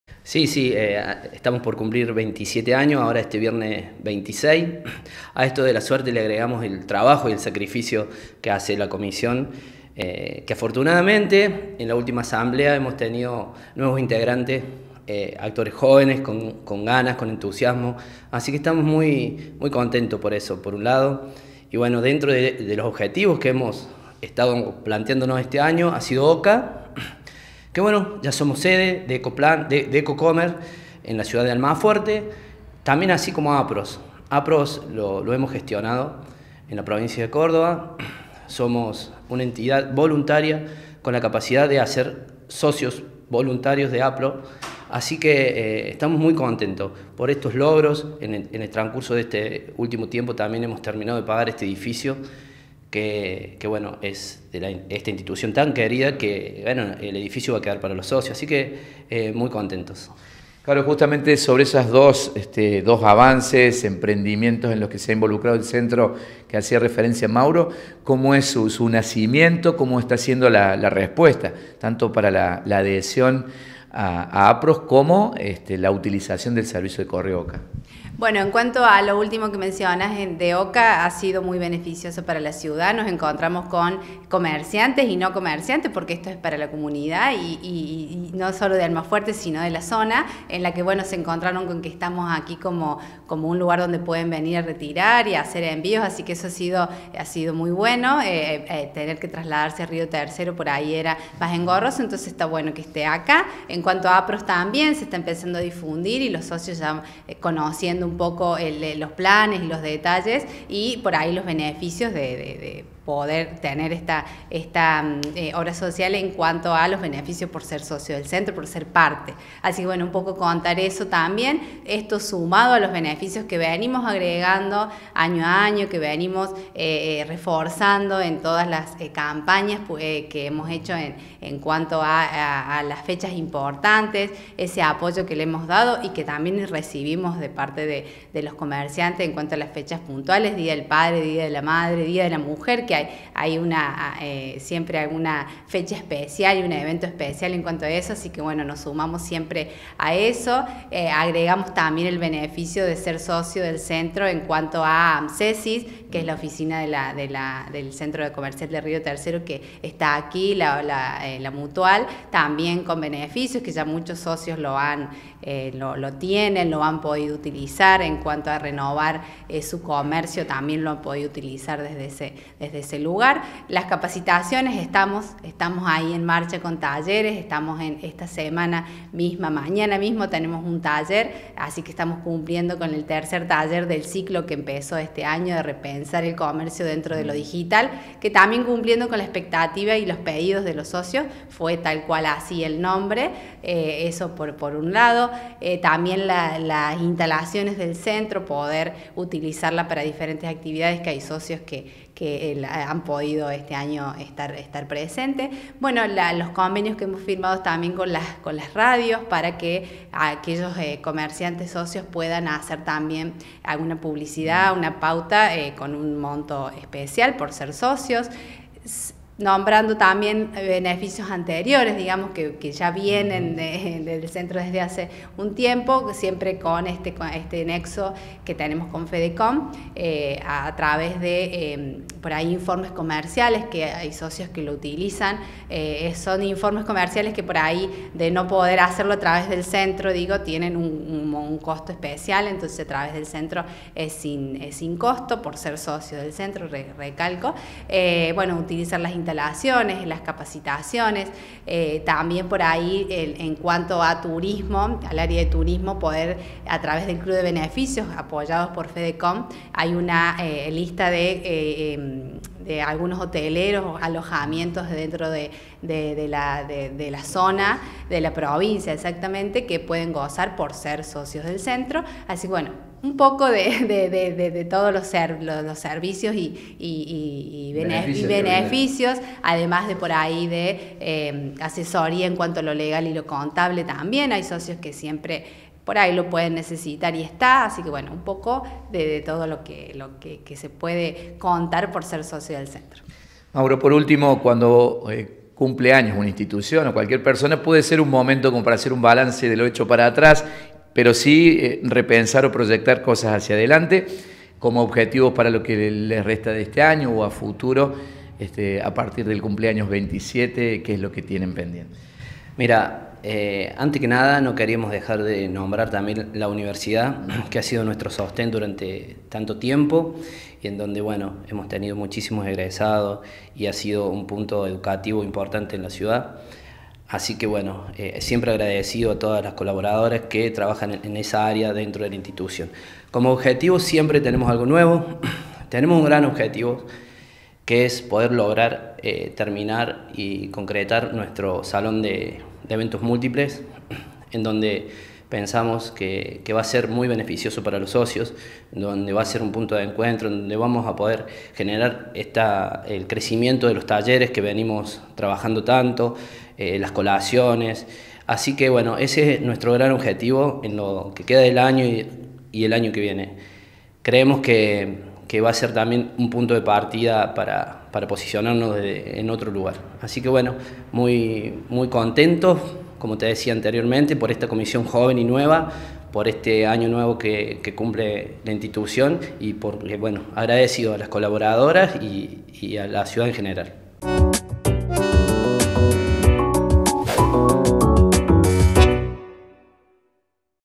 ANIVERSARIO DEL CENTRO
El Centro Comercial celebrará sus 27 años de vida. Sus autoridades
hicieron un balance de los últimos años y adelantaron futuros
proyectos